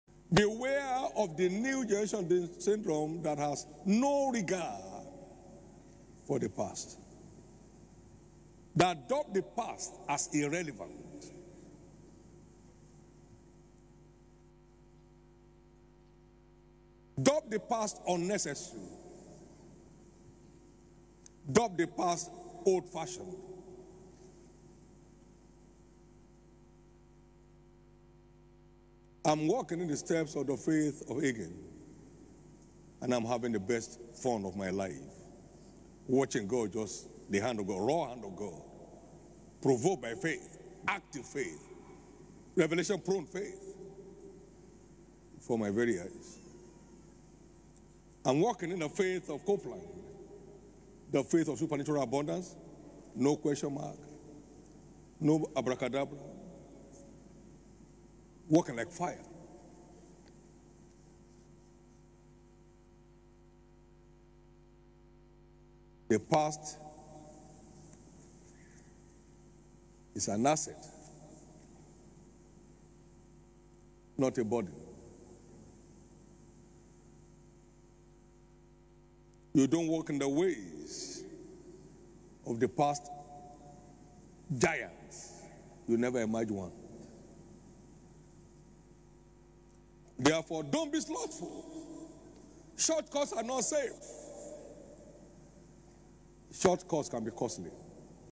Bishop Oyedepo's message to the youth:
BISHOP OYEDEPO TO THE YOUTH:

BEWARE OF THE NEW GENERATION SYNDROME THAT HAS NO REGARD FOR THE PAST. THAT DUBS THE PAST AS IRRELEVANT, UNNECESSARY, AND OLD FASHIONED.

THE PAST IS AN ASSET, NOT A BURDEN. YOU DO NOT WALK IN THE WAYS OF THE PAST GIANTS, YOU NEVER EMERGE ONE

THE BENEFITS OF THE PAST ARE UNFATHOMABLE!

I AM WALKING IN THE FAITH OF HAGIN AND COPELAND